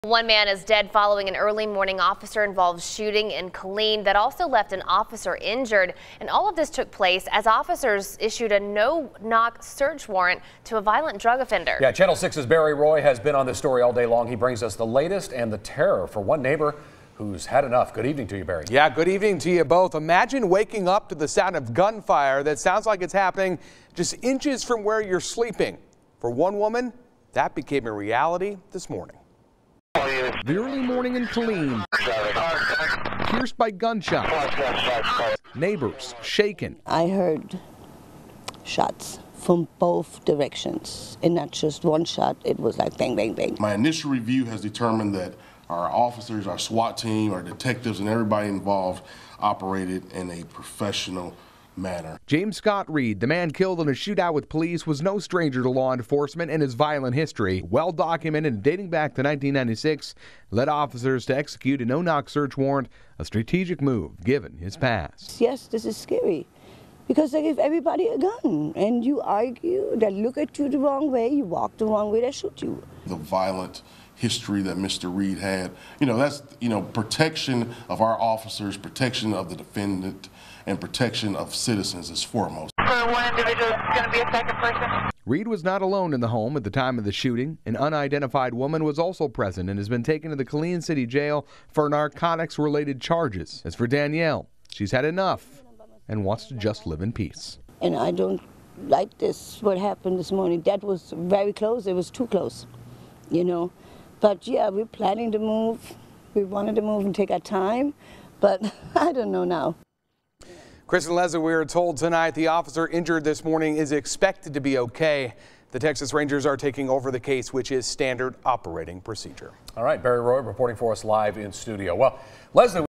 Officer-involved shooting in Killeen leaves one man dead
Officers were serving a no-knock warrant for James Reed at the home before the shooting, police said. Reed was a known drug dealer who was "always armed" and had a criminal history of terroristic threats, robbery and selling drugs, police said.